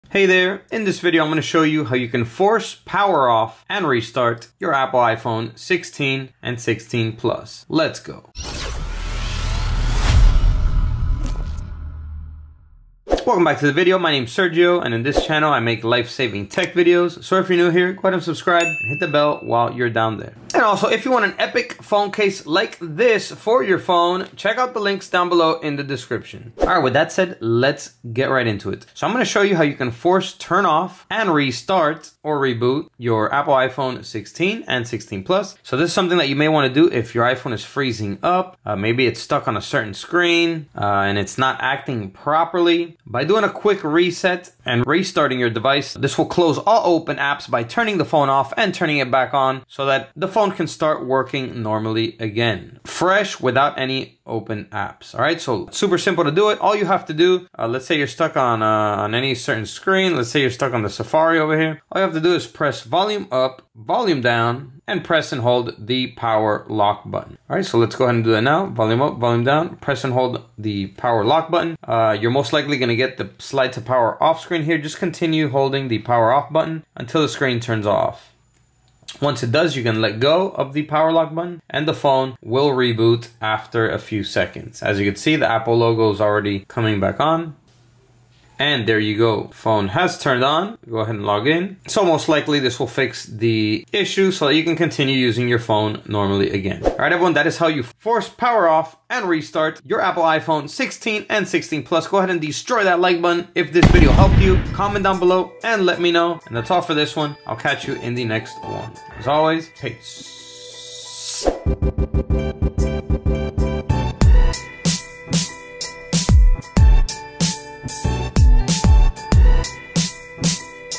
How to Force RESTART/REBOOT iPhone 16 & 16 Plus - Frozen Screen Fix
This is a video on how to force turn off and restart your iPhone 16 and 16 Plus. Soft reset to force turn off and reboot your iPhone 16 when it gets stuck or frozen on a screen.

📱 TECH STUFF
iPhone Parts
Repair Tools
Best MacBook Accessory
Unlock Your iPhone